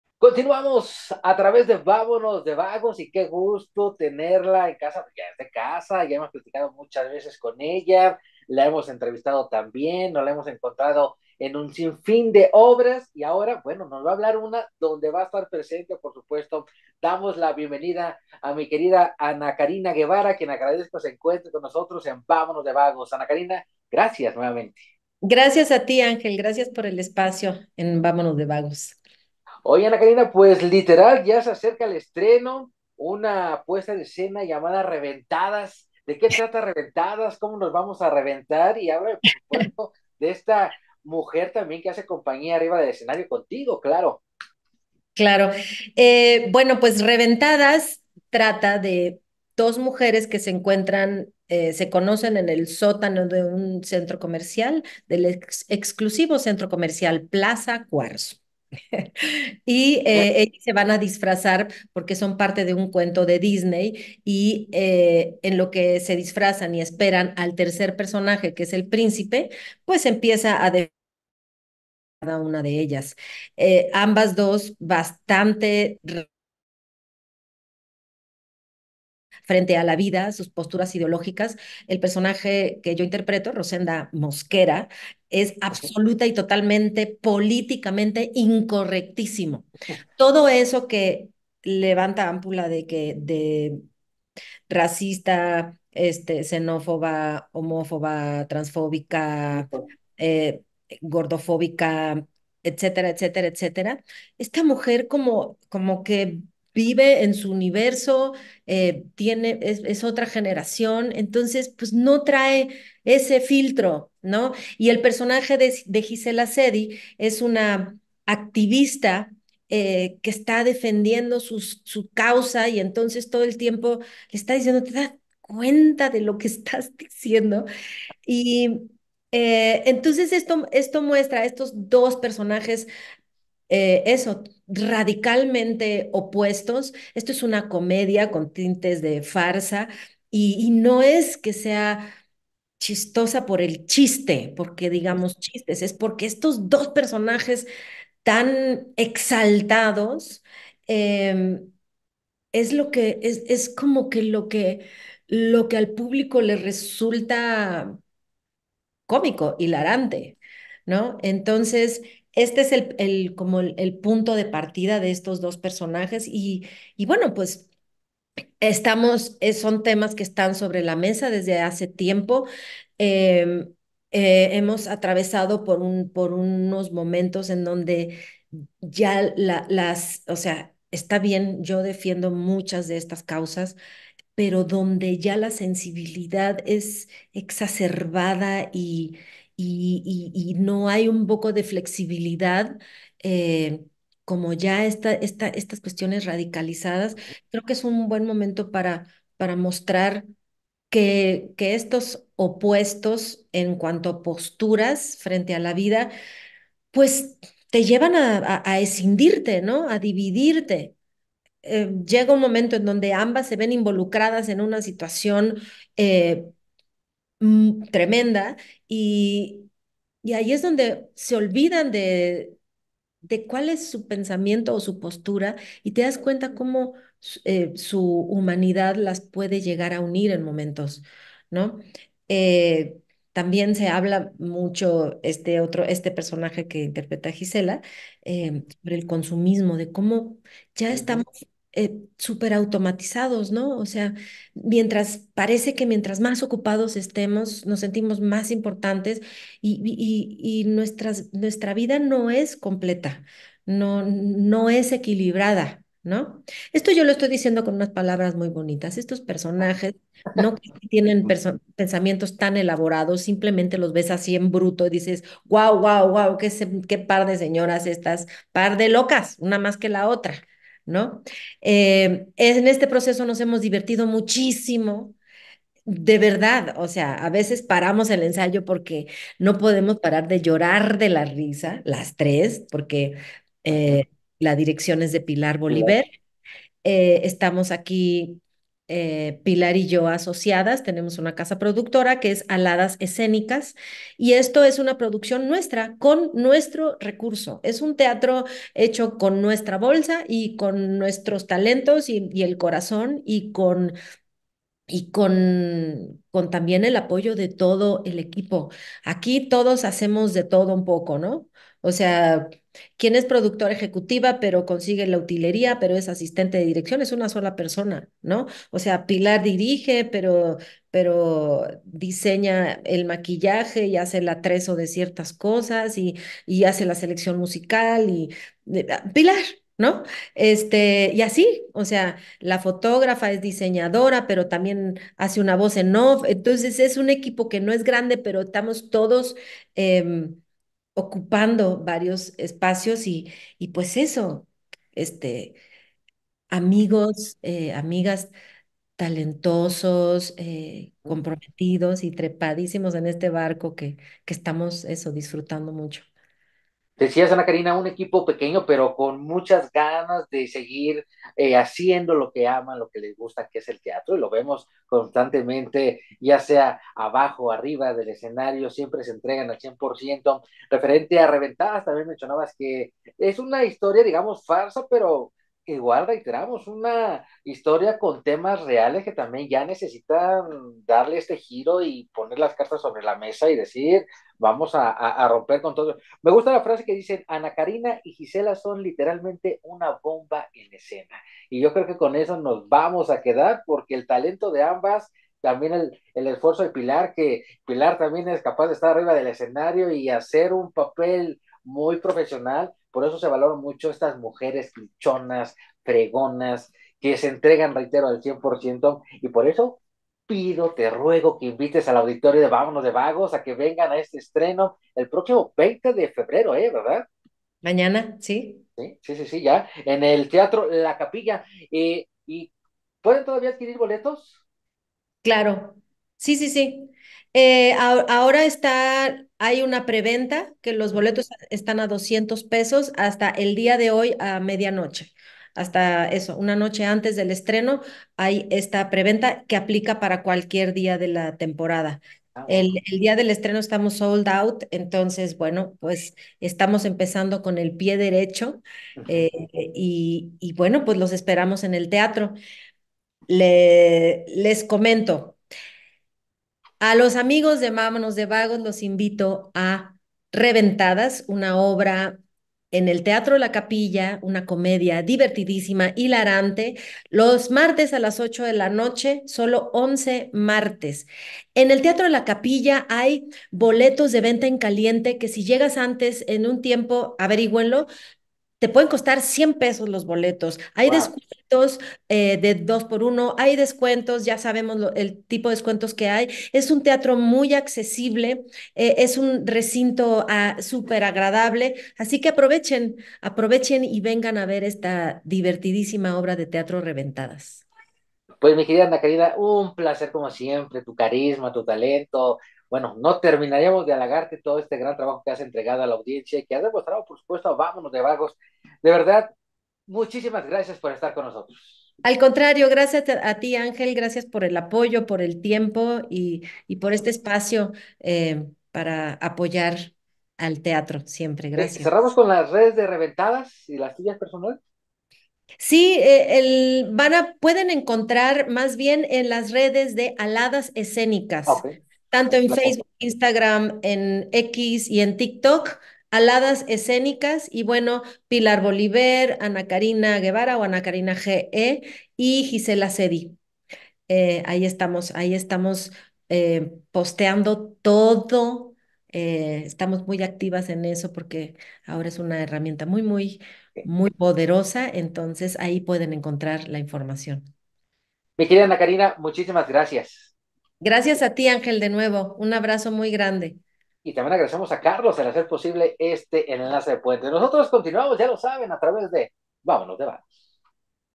Reventadas
🎙️ Platicamos con la actriz Ana Karina Guevara sobre reventadas con Gicela Sehedi 👥

🎭 La Capilla Teatro / Boletópolis
         Dirección Pilar Boliver * Aladas Escenicas